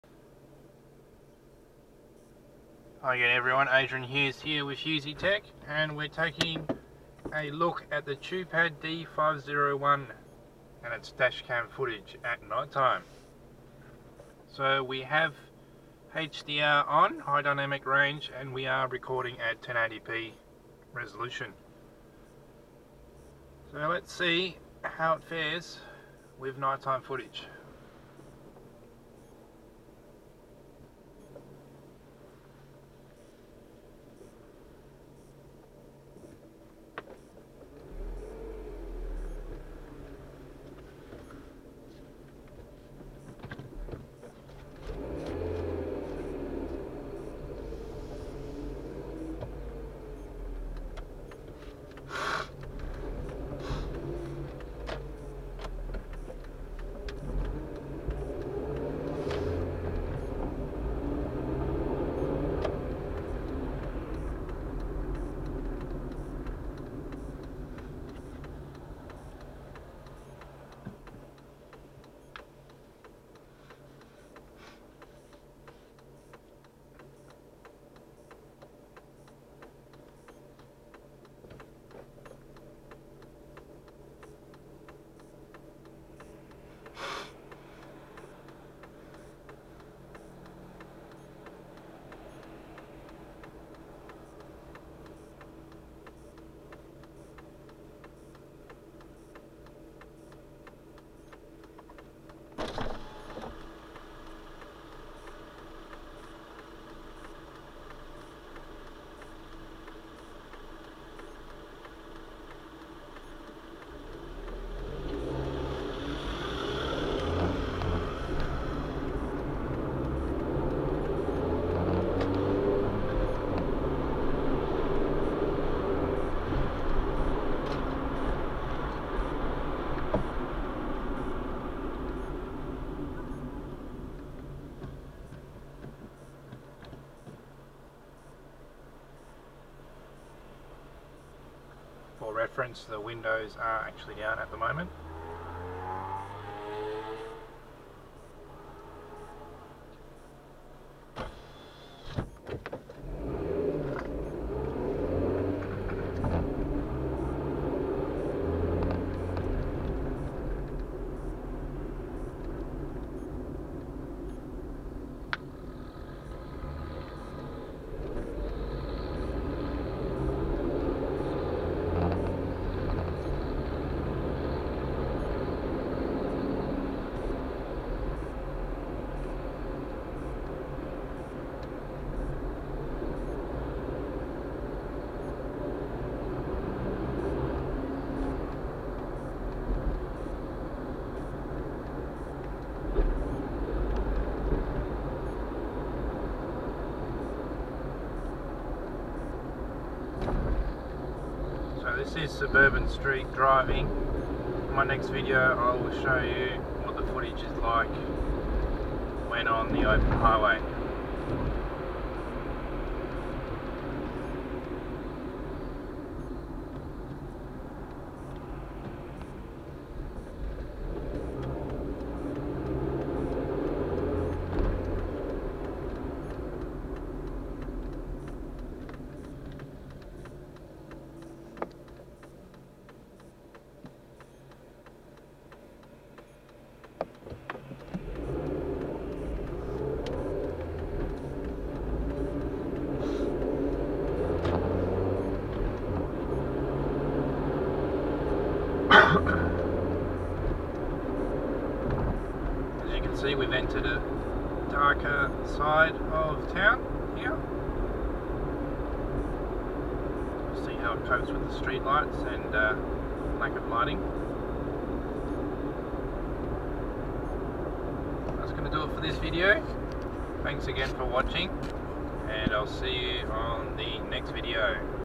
Chupad D501 dashcam sample (night-time footage at 1080p, WDR/HDR turned on)
Here is footage from the Chupad D501 dashcam, night-time at 1080p with WDR/HDR turned on.

It features full 1080p HD recording with 170 degrees field of view, utilising a Sony IMX322 sensor and Novatek NT96658 chipset.